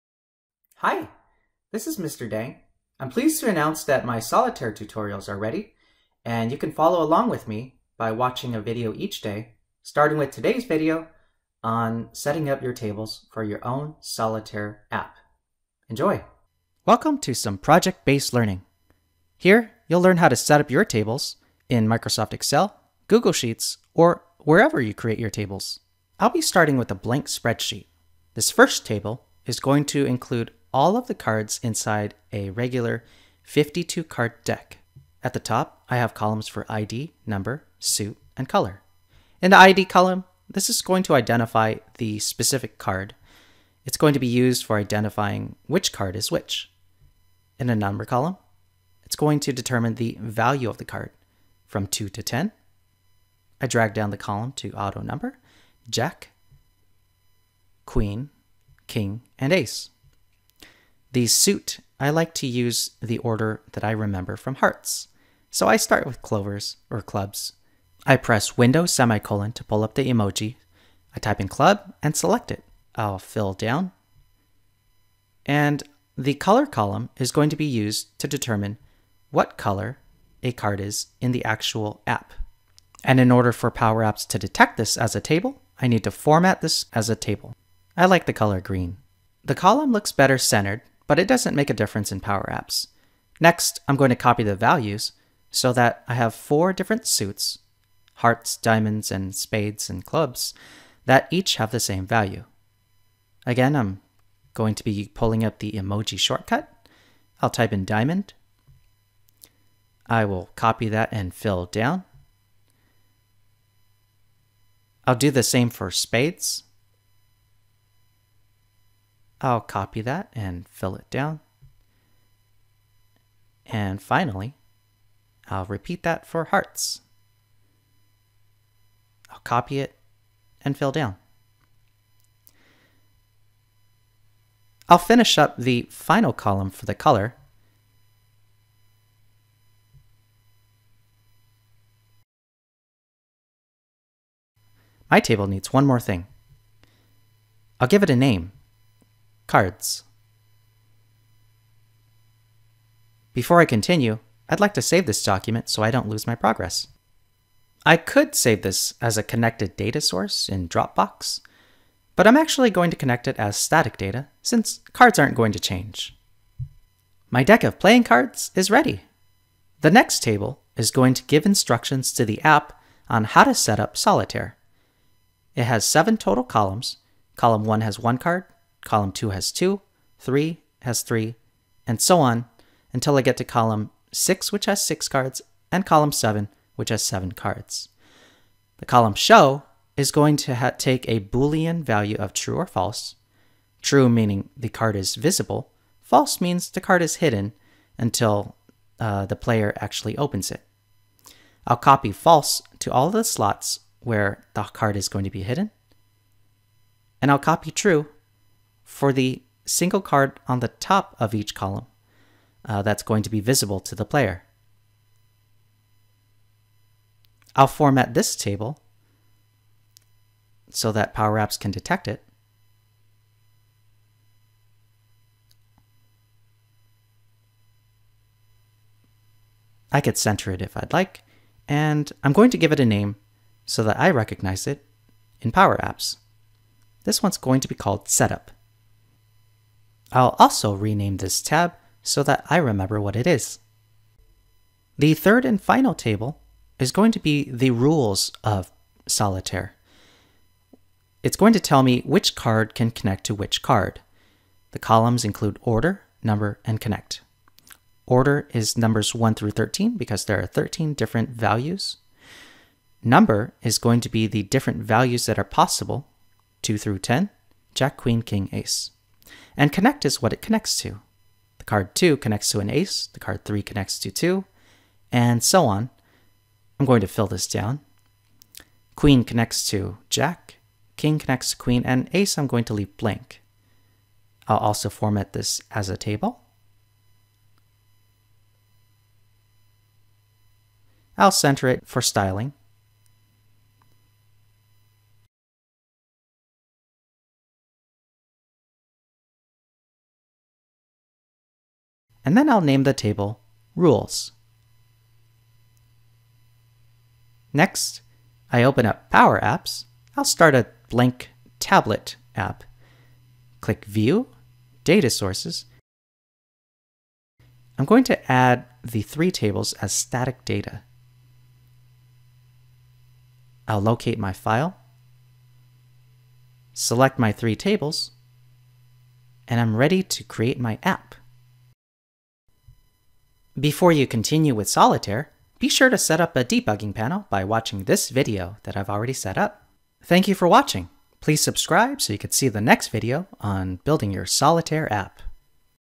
Solitaire - 1 - Setting up Tables [PowerApps]
My tutorial for creating your own Solitaire app is ready to roll out. I will release a new step each day.

My students learn best through project-based learning, so this is a worthwhile project that involves skills useful in creating other apps as well.